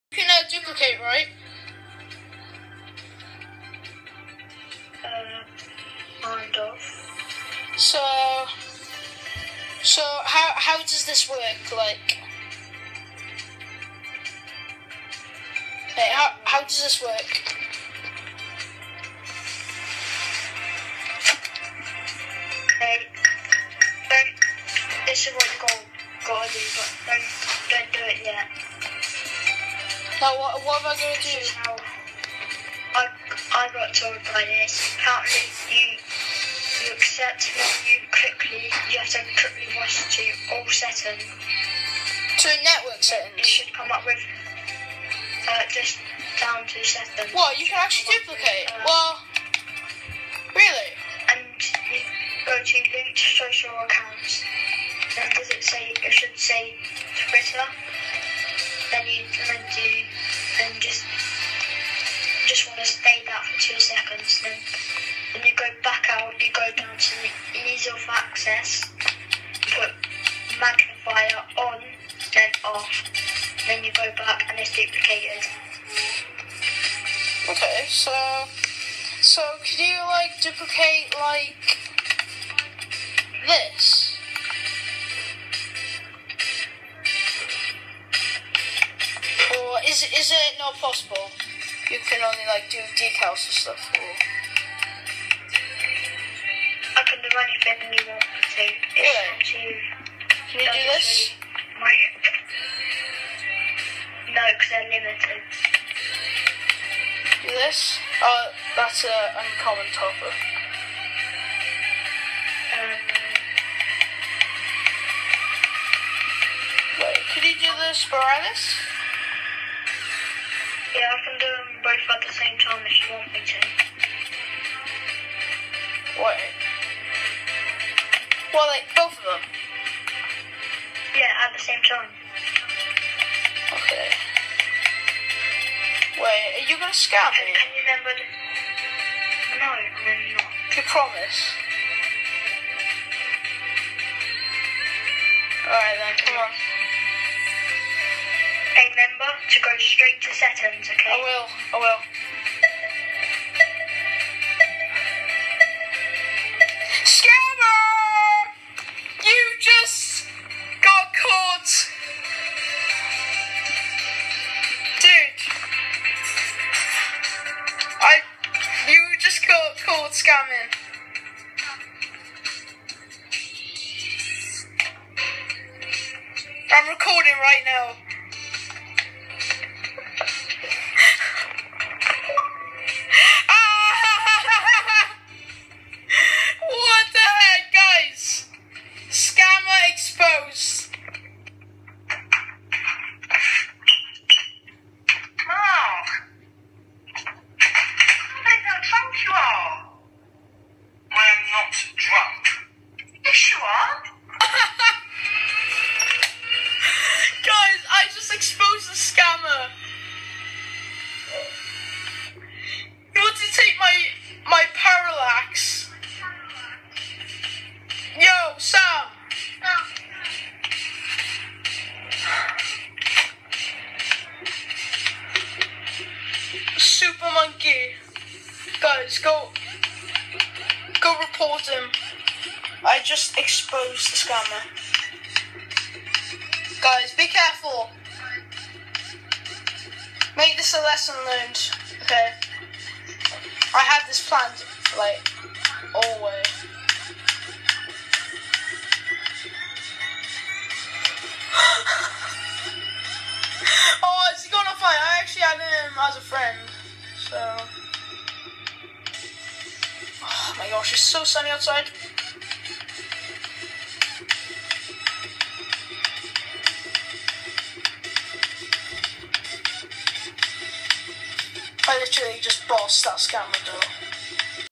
*SCAMMER EXPOSED* SCAMMER TRIES TO SCAM ME FOR MY PARRALAX!!!!! 😱😱😱😱😱
Hey guys please report this SCAMMER scammer is called SuperMonkey2003